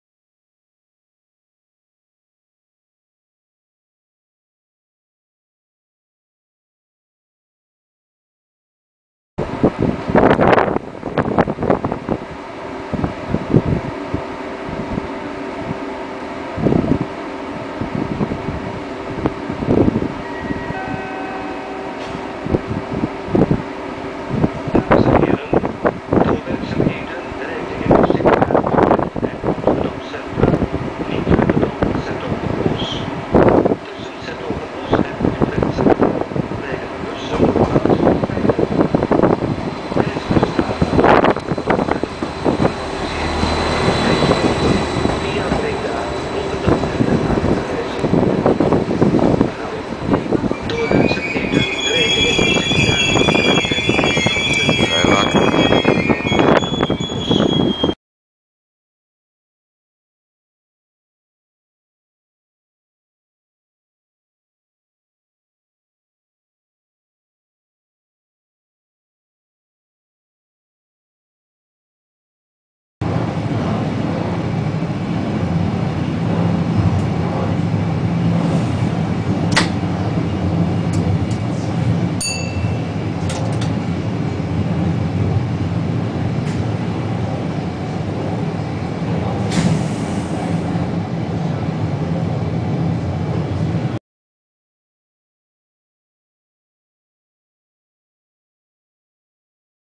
Veolia GTW
My first ride on a Veolia GTW, including a cabride!!
PS Don´t listen to ´vuilak´, it was just said because the train didn´t stop entirely as I wanted him to, nothing against the train nor the engineer!

(Partly in English now! I´m working on it)